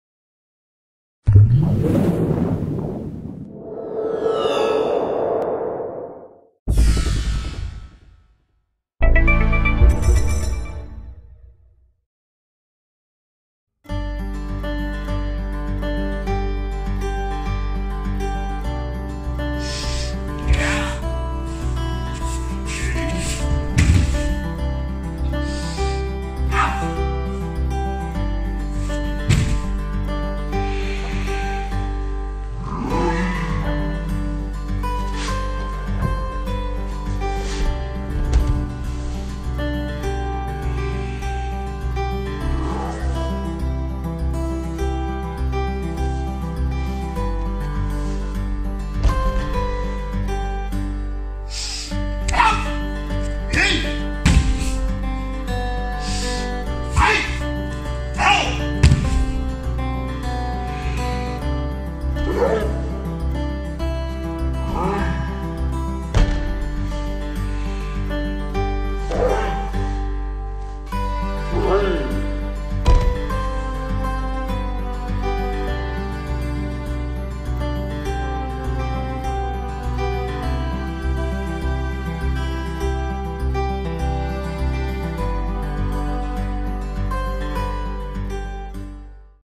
TACHIDORI aihanmi KOKYUNAGE 3 - Marco Rubatto
Forma di base (kihon) e fluida (ki no nagare) di "presa della spada stessa guardia proiezione respiratoria 3" (tachidori kokyunage3)

Basic (kihon) and fluid (ki no nagare) form of "sword grip same guard respiratory throw 3" (tachidori kokyunage3)